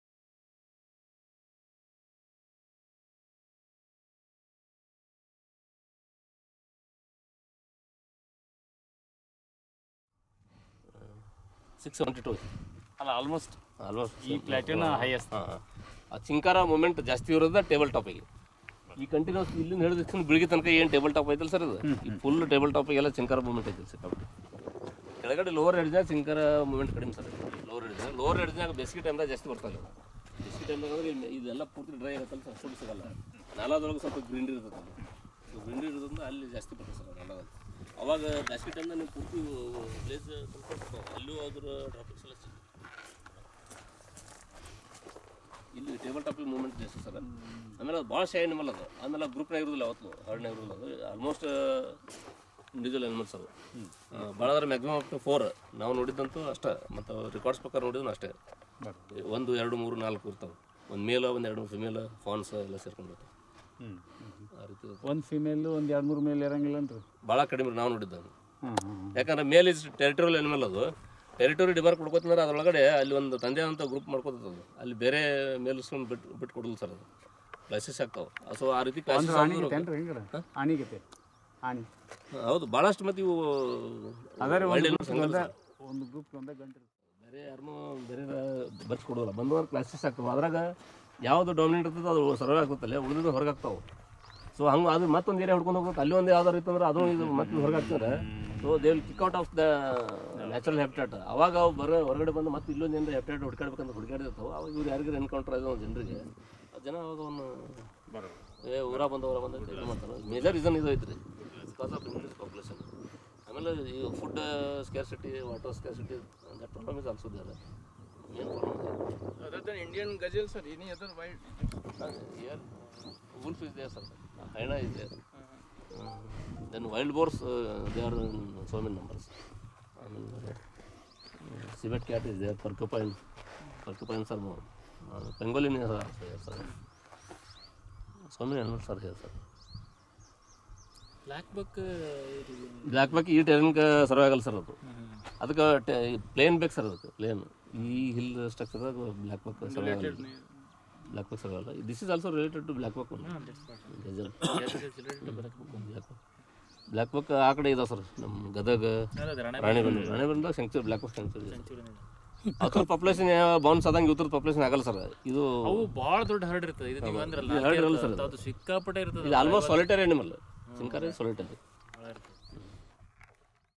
CHINKARA WILD LIFE CENTURY
DR.V.B.HOSAGOUDAR MADE SURVEY OF CHINKRA WILDLIFE CENTURY IN YEDALLI,BILGI.TQ,BAGALKOT.DIST,KRNTAKA